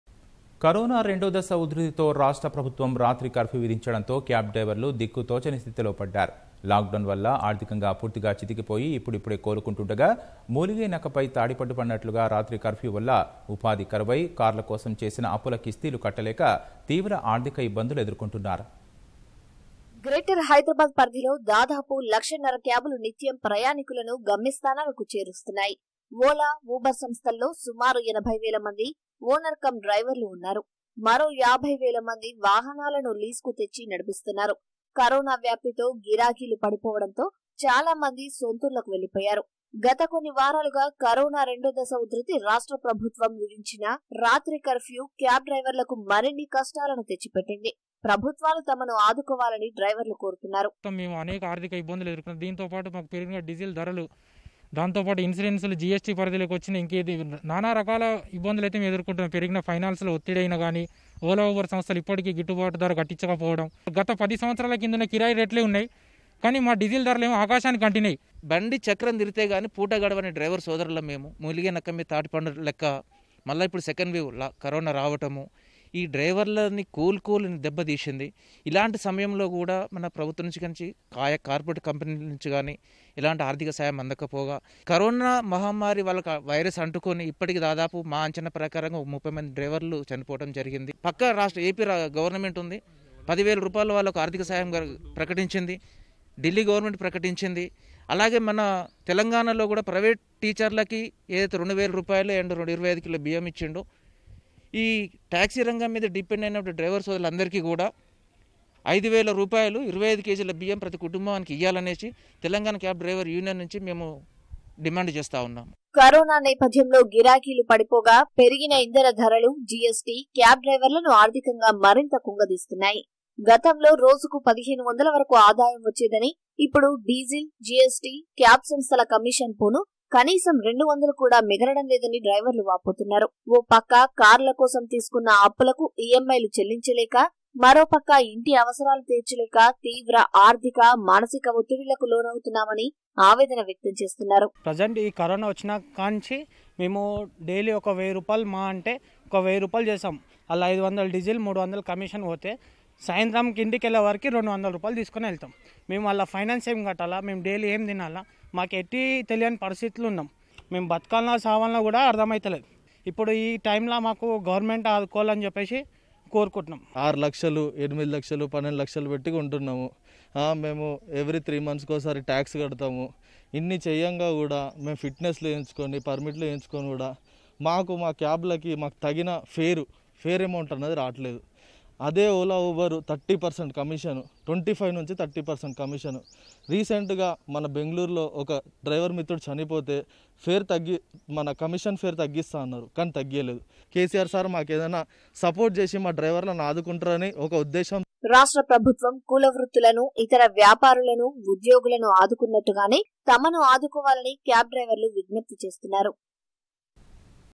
Story on Cab Drivers Woes | Amid Covid Surge, Petrol Price Hike & Other Taxes
కరోనా రెండో దశ ఉద్ధృతితో రాష్ట్ర ప్రభుత్వం రాత్రి కర్ఫ్యూ విధించడంతో క్యాబ్  డ్రైవర్లు దిక్కుతోచని స్థితిలో పడ్డారు. లాక్ డౌన్ వల్ల ఆర్థికంగా పూర్తిగా చితికిపోయి ఇప్పుడిప్పుడే కోలుకుంటుండగా...మూలిగే నక్కపై తాటిపండు పడ్డట్టు రాత్రి కర్ఫ్యూ వల్ల ఉపాధి కరువై, కార్ల కోసం చేసిన అప్పుల కిస్తీలు కట్టలేక...తీవ్ర ఆర్థిక ఇబ్బందులు ఎదుర్కొంటున్నారు.